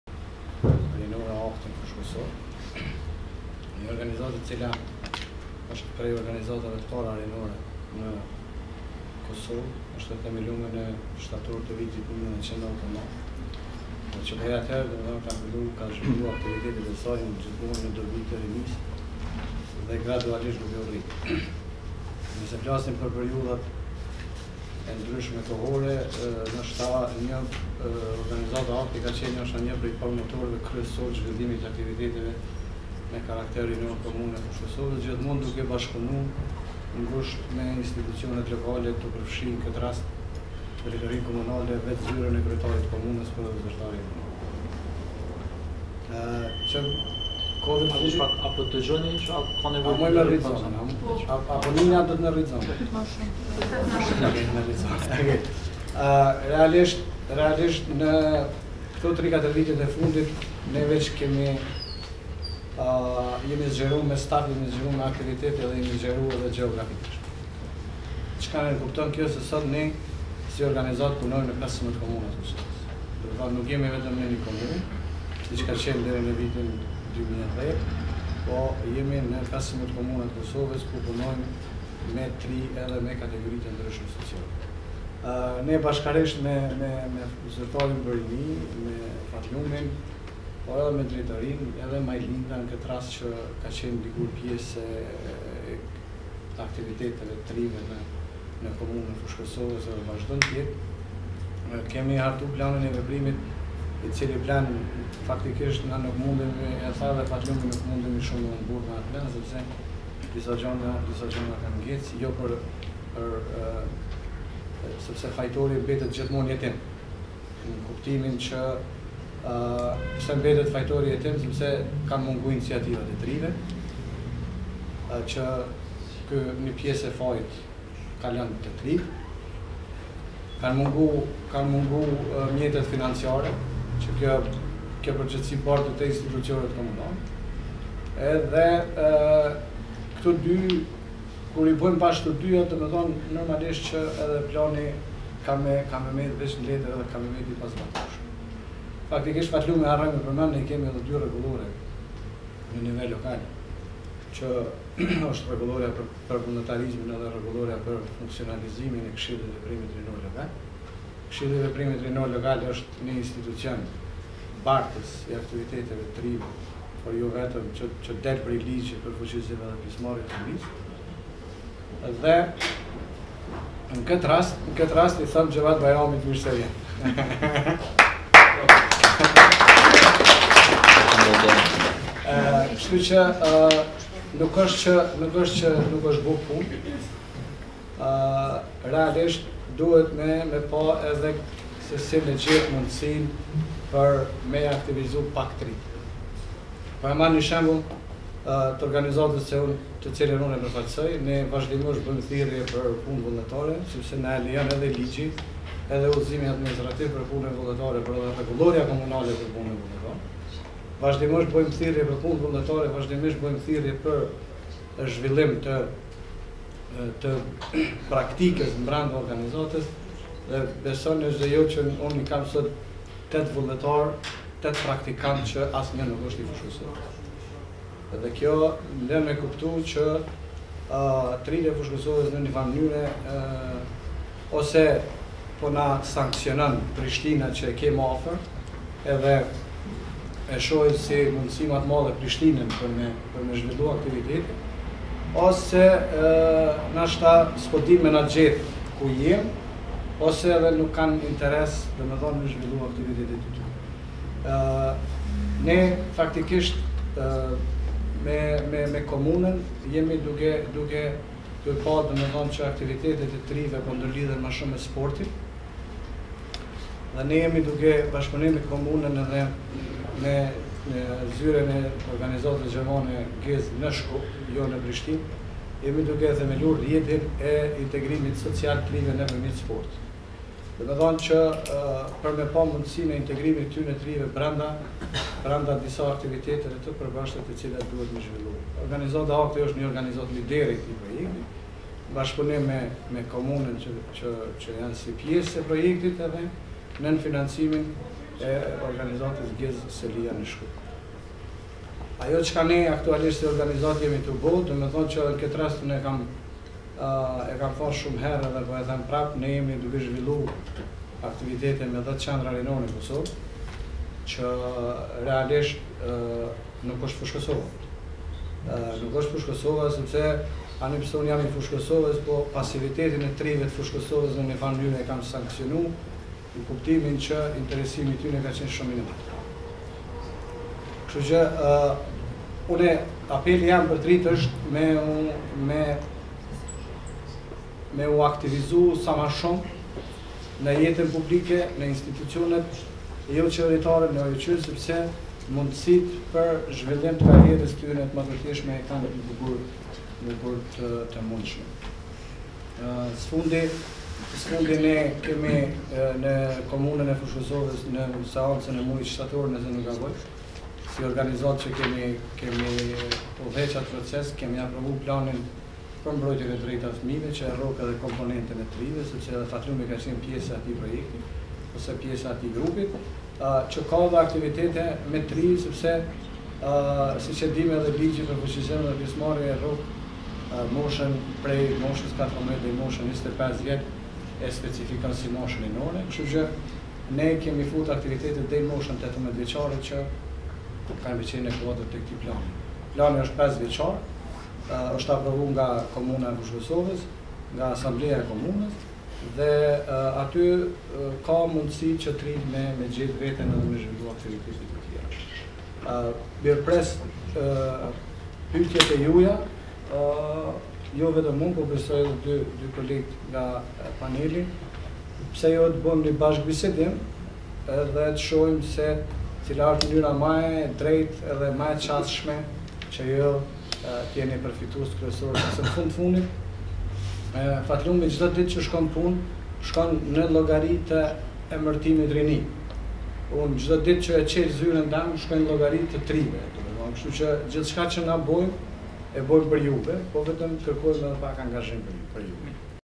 Rexhep Gojnovci, Drejtor Ekzekutiv, OJQ AKTI, sallonet e së martes
Rexhep Gojnovci, Drejtor Ekzekutiv, OJQ AKTI, flet për: Iniciativat e Rinisë: Si të rriten dhe ku të gjejnë përkrahje?